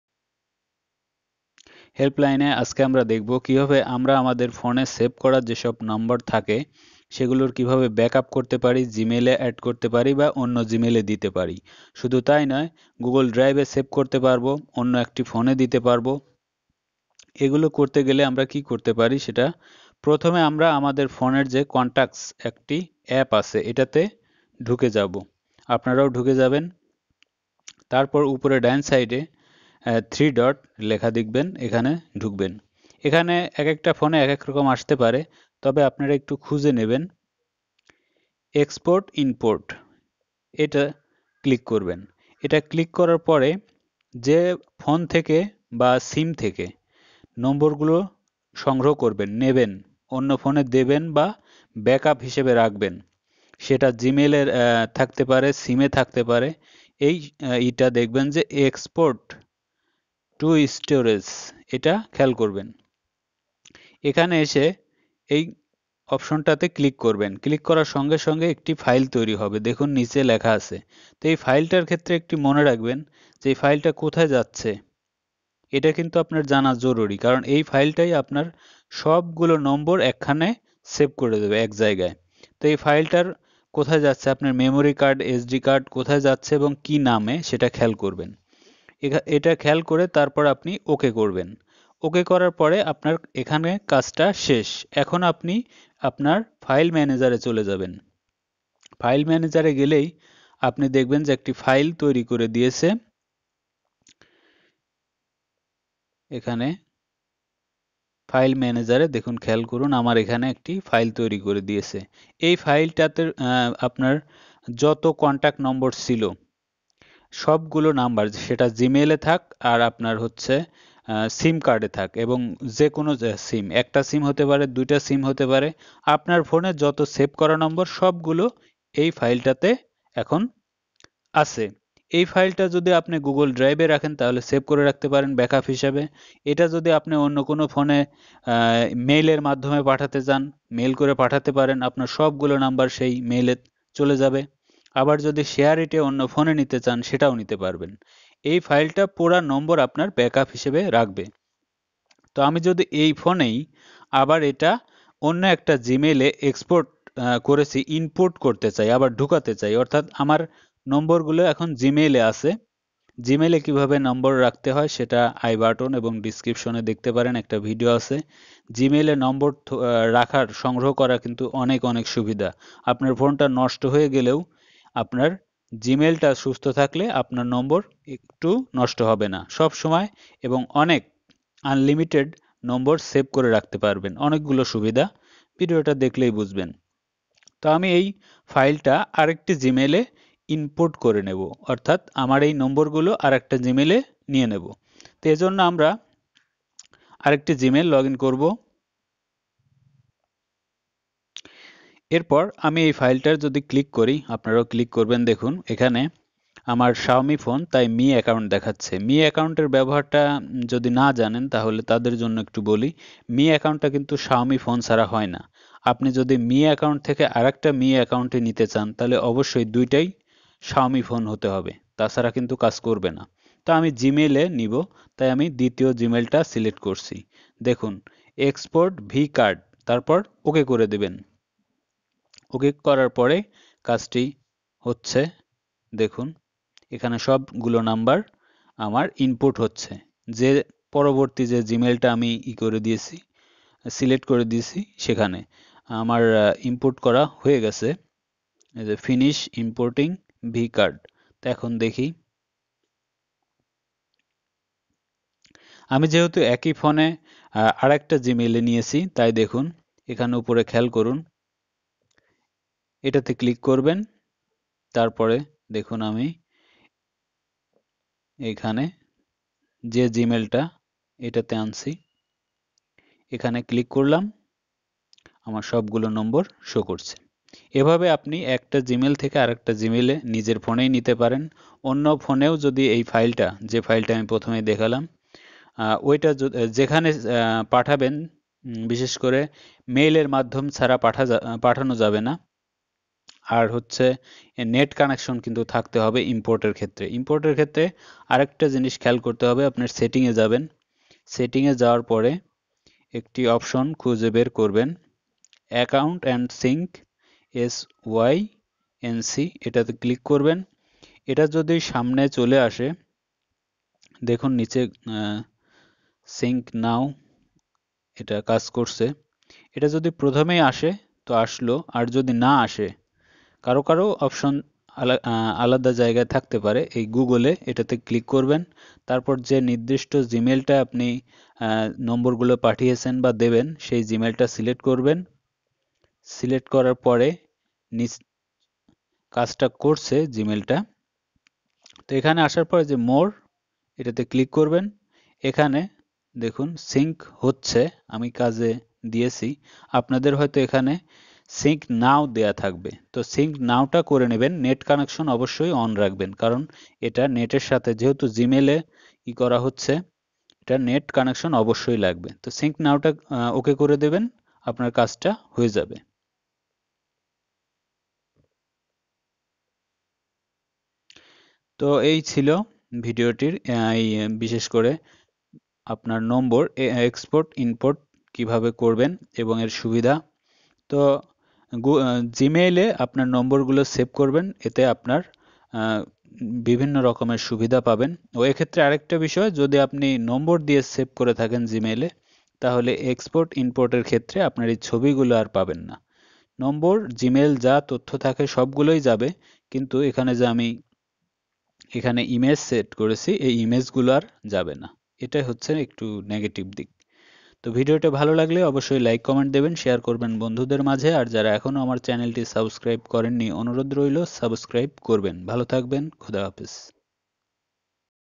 How to import/export contacts number on Gmail | Keep backup contact number in Gmail || Helpline HKFY
Now you can import/export your contacts number on Gmail. Import contacts from gmail to android phone,just with single click we can sync the contacts of gmail into your android phone || Helpline HKFY

Follow The Steps:
Step 1
Press the menu button on the Android device
and tap “Settings”
Step 2
Tap “Accounts and sync”
Step 3
Tap the “Add account” button.
Step 4
Tap “Google.”
Step 5
Tap the “Next” button.
Step 6
Tap “Sign in” and enter your Gmail login
information. If you don’t already have a Gmail
account, tap the “Create” button instead and fill
in the form provided.
Step 7
Tap the “Sign in” button. The device will
download the Gmail account’s information.
Step 8
Tap the “Sync Contacts” option.
Step 9
Tap the “Finish” button.

    ≈≈≈≈≈≈≈≈≈≈≈≈↓ Follow Me ↓≈≈≈≈≈≈≈≈≈≈≈≈